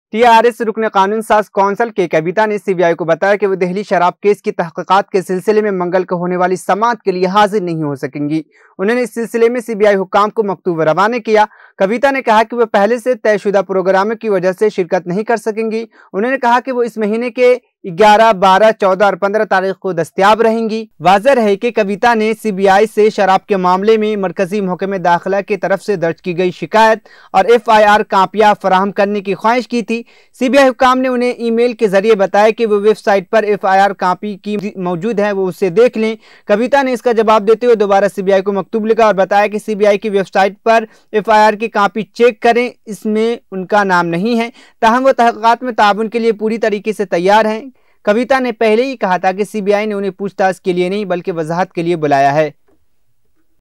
Kavita's letter to CBI. She will not be able to appear for the hearing tomorrow || URDUTVNEWS
Kavita's letter to CBI. She will not be able to appear for the hearing tomorrow || URDUTVNEWS || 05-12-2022